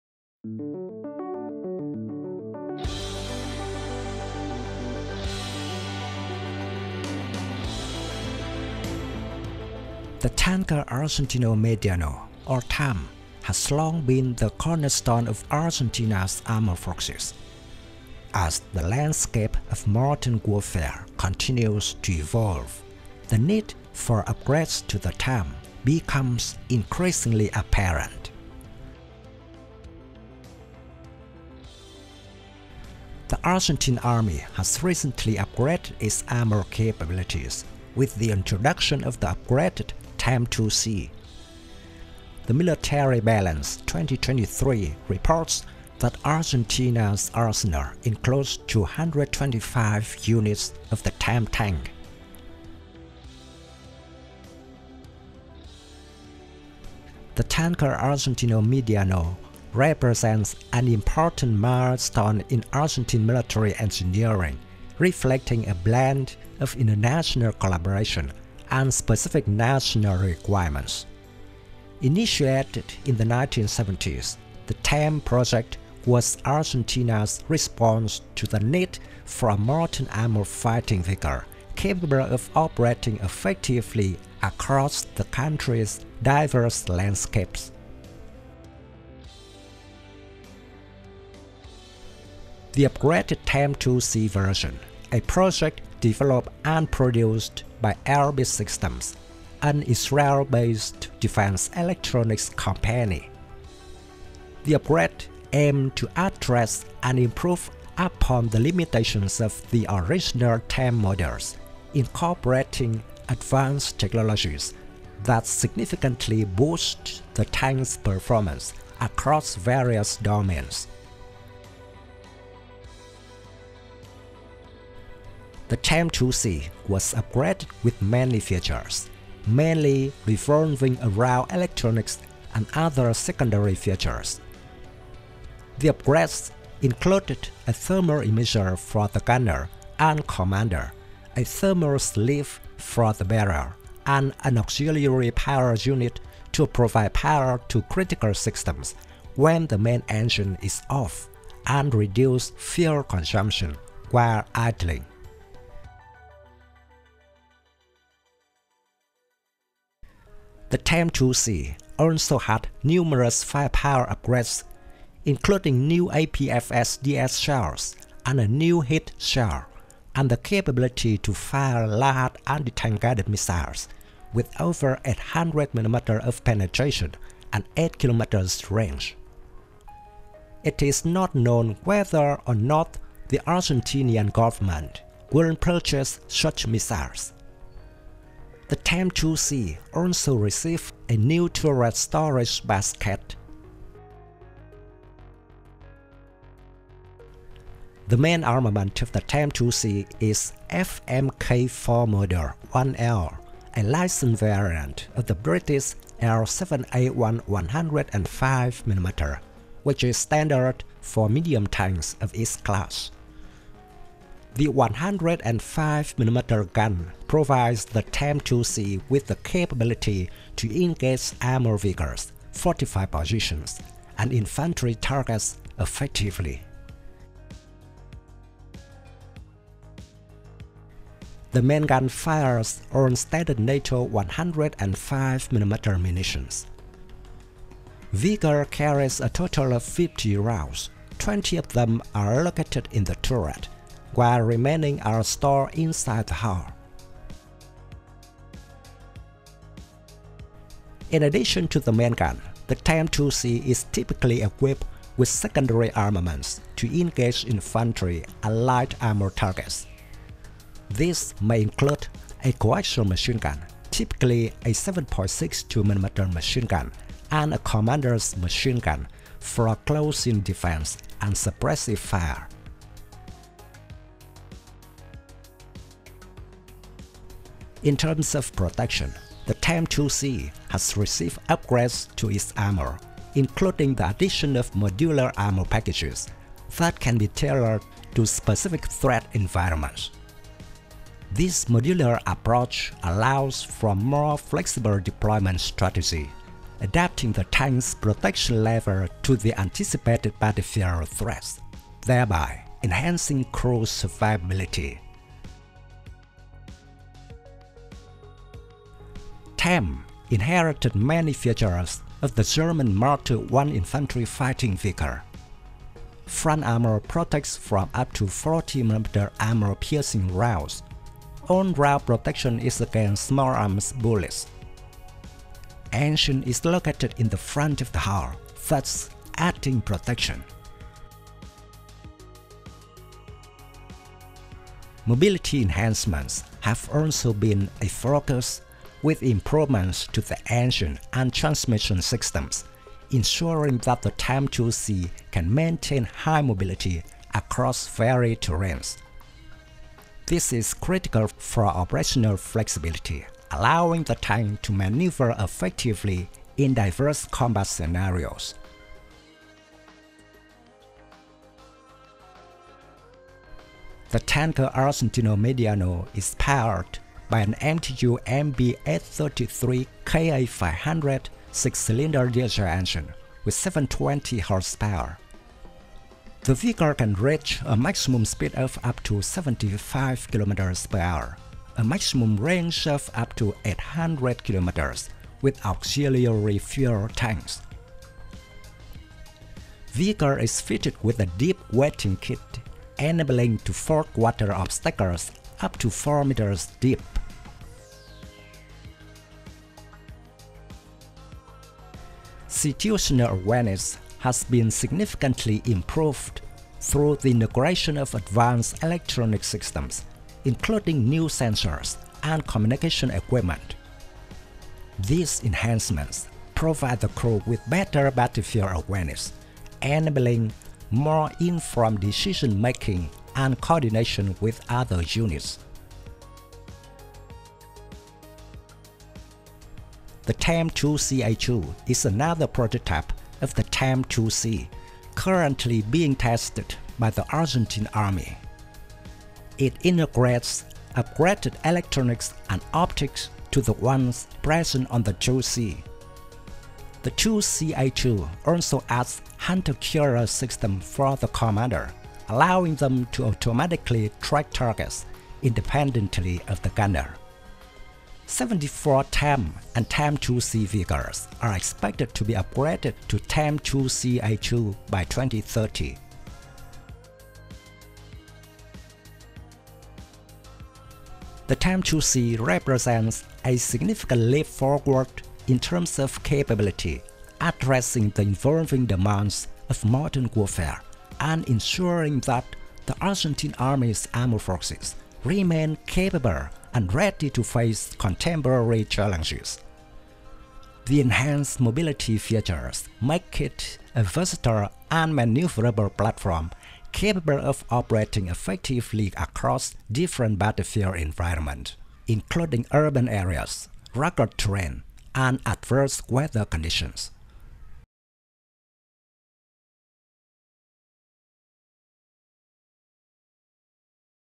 TAM 2C: Argentina's Next-Gen Medium Tank Upgrade!
Discover the TAM 2C, Argentina's latest advancement in armored warfare! Join us as we delve into the remarkable enhancements of the TAM 2C, the newest addition to the Argentine army's arsenal. Explore how this significant upgrade transforms the TAM medium tank line, delivering enhanced capabilities and firepower to bolster Argentina's defense strategy. Don't miss out on this in-depth look at the TAM 2C and its pivotal role in modernizing Argentina's armored forces.